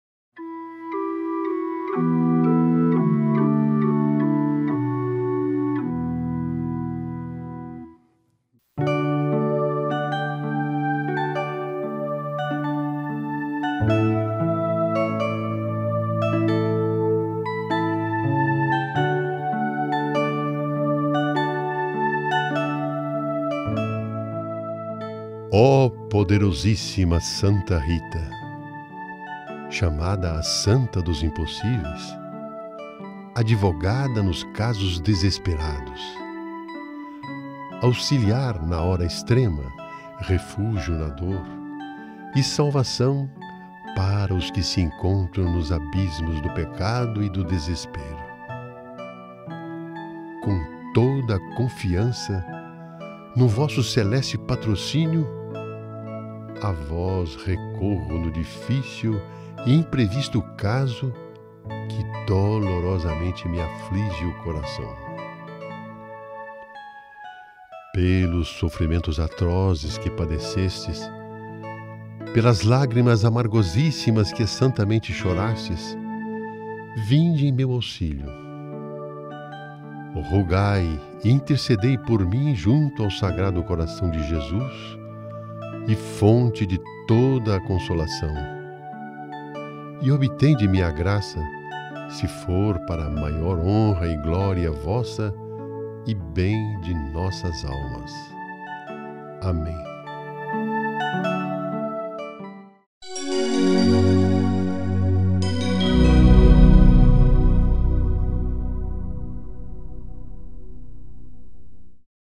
Oração Santa Rita Cássia-TV ARAUTOS
A TV Arautos, dos Arautos do Evangelho, apresenta um novo programa: Orações. É através da oração que nossa alma chega até Deus. Ela é a respiração da alma, ela é no nosso alívio nos sofrimentos, em nossas dores, consolo para nós. Devemos orar sempre, sem desânimo. Pois, “Quem reza se salva, quem não reza se condena!” Afirma o grande Santo Afonso Maria de Ligório.

Ó poderosa Santa Rita, chamada a Santa dos impossíveis, advogada nos casos desesperados, auxiliar na hora extrema, refúgio na dor, e salvação para os que se encontram nos abismos do pecado e do desespero, com toda a confiança, no vosso celeste patrocínio, a vós recorro no difícil e imprevisto caso que dolorosamente me aflige o coração.

Pelos sofrimentos atrozes que padecestes, pelas lágrimas amargosíssimas que santamente chorastes, vinde em meu auxílio. Rogai e intercedei por mim junto ao Sagrado Coração de Jesus, fonte de toda a consolação e obtende-me a graça, se for para a maior honra e glória vossa e bem de nossas almas.

Amém.